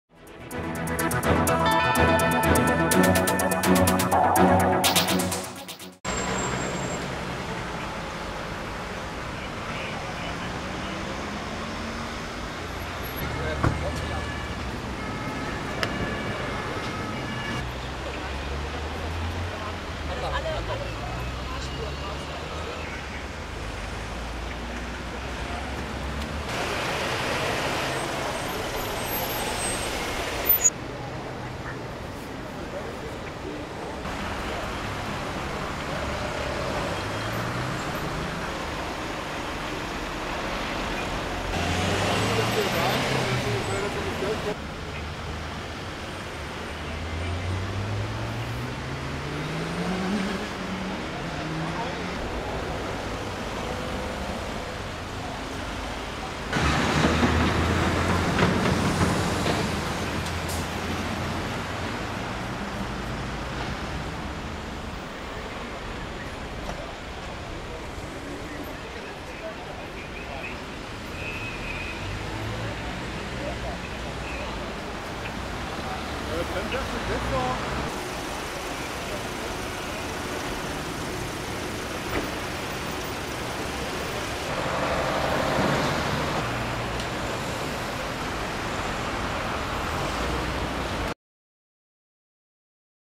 Polizei verunglückt bei Einsatzfahrt in Essen
Am heutigen Nachmittag ein Einsatzfahrzeug der Essener Polizei verunglückt. Das Polizeiauto stieß frontal mit einem anderen Pkw (Peugeot) an einer Kreuzung zusammen. Eine Person aus dem Peugeot wurde bei dem Unfall verletzt. Über die Schwere der Verletzung kann noch nichts gesagt werden. Weitere Einsatzkräfte sowie die Essener Feuerwehr wurden zur Einsatzstelle beordert. Die B224 musste für die Dauer des Einsatzes in eine Fahrtrichtung gesperrt werden. Die genaue Unfallursache ist zurzeit noch ungeklärt.